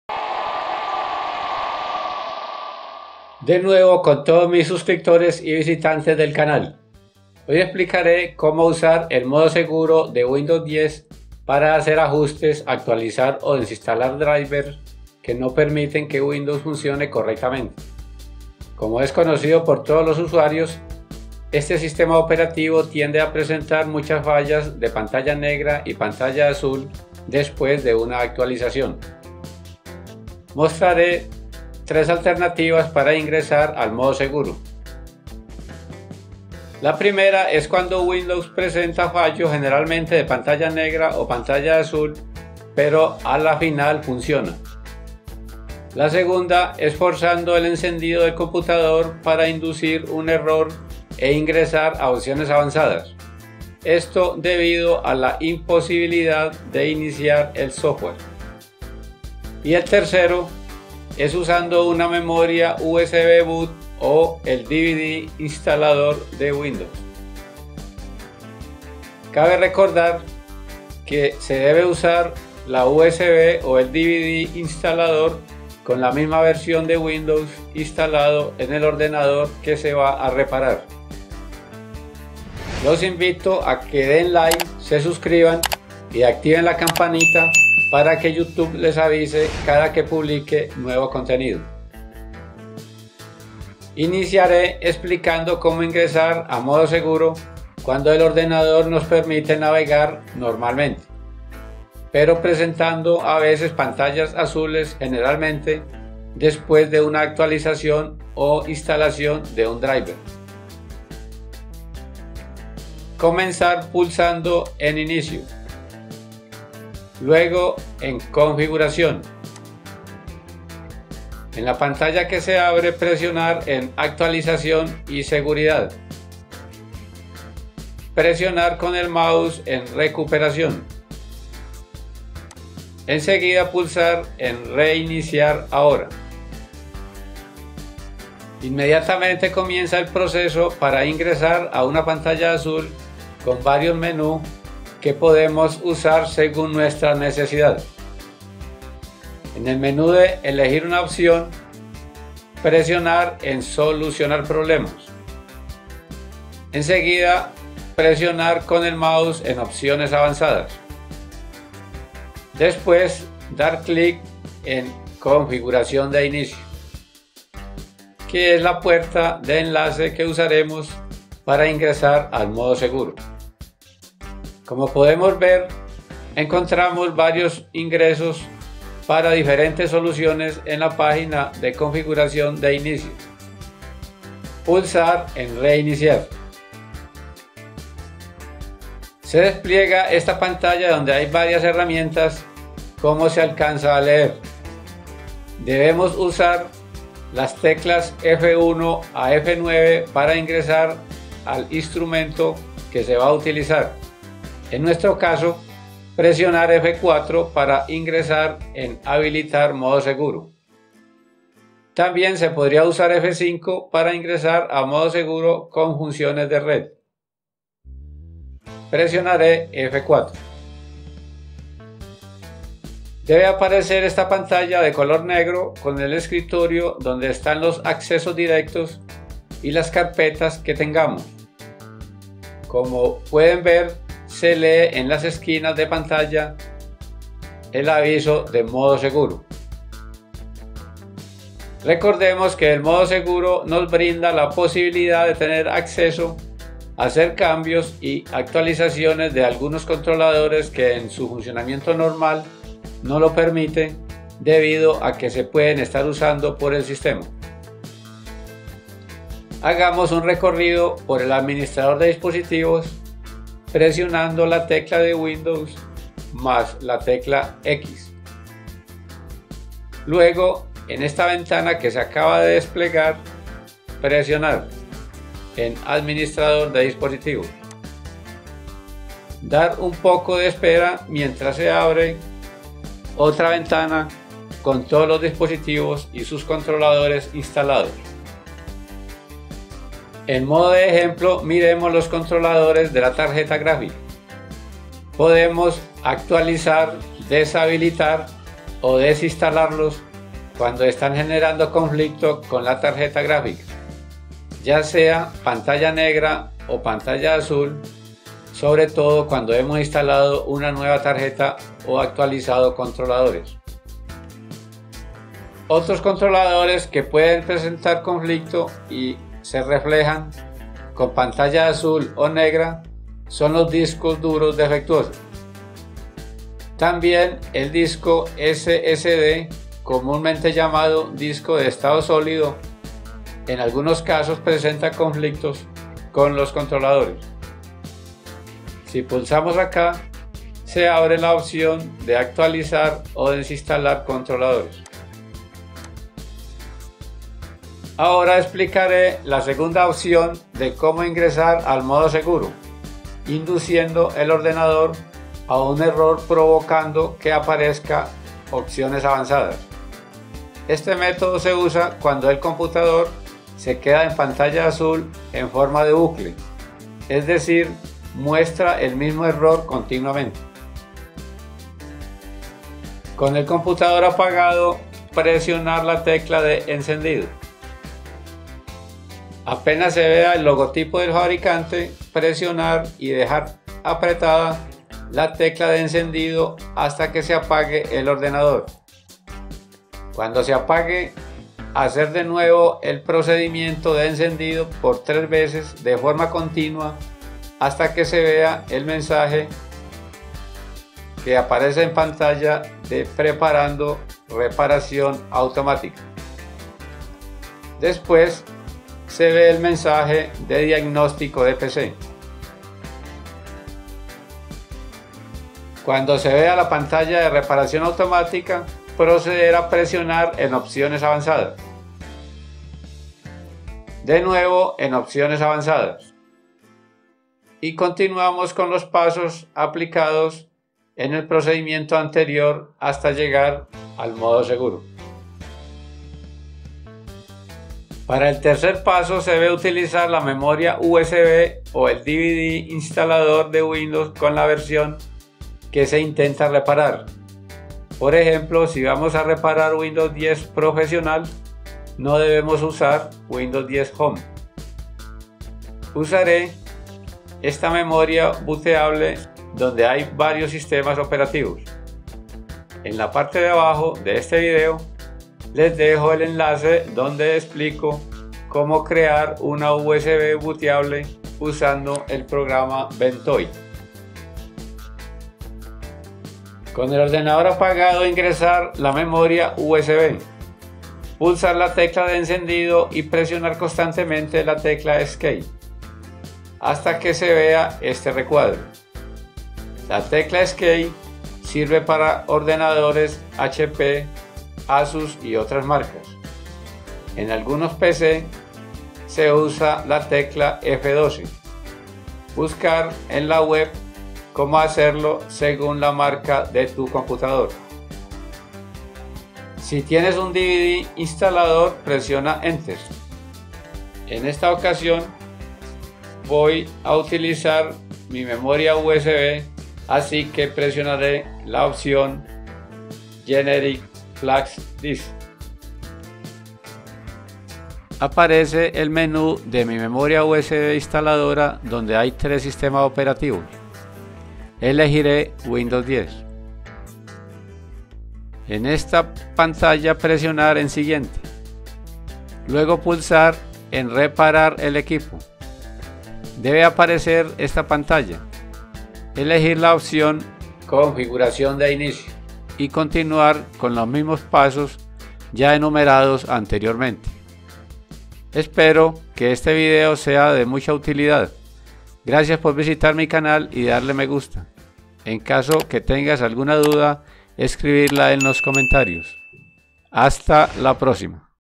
Cómo Ingresar al MODO SEGURO en Windows 10 y 11 - Guía Completa
Hoy explicaré como usar el “Modo Seguro” de Windows 10 para hacer ajustes, actualizar o desinstalar drivers que no permiten que Windows funcione correctamente.
Como es conocido por todos los usuarios, este sistema operativo tiende a presentar muchas fallas de pantalla negra y pantalla azul, después de una actualización.

Mostraré tres alternativas para ingresar al “Modo seguro”.